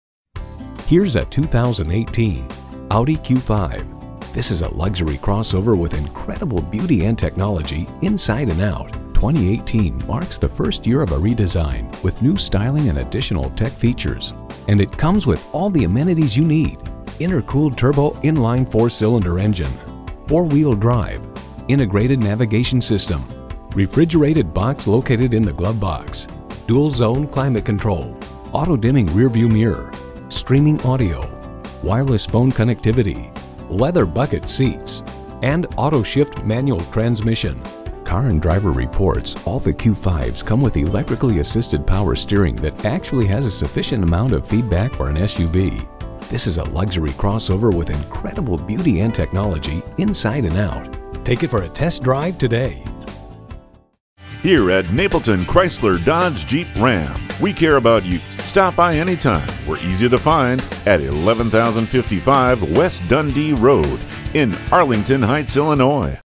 Used 2018 Audi Q5 Arlington Heights, IL
Year: 2018
Make: Audi
Model: Q5
Trim: 2.0T
Engine: 2.0L TFSI
Transmission: 7-Speed Automatic S tronic
Color: Black
Interior: Black
Mileage: 43019
Stock #: PJ3009
VIN: WA1ANAFY7J2172552

Address:
1155 W Dundee Rd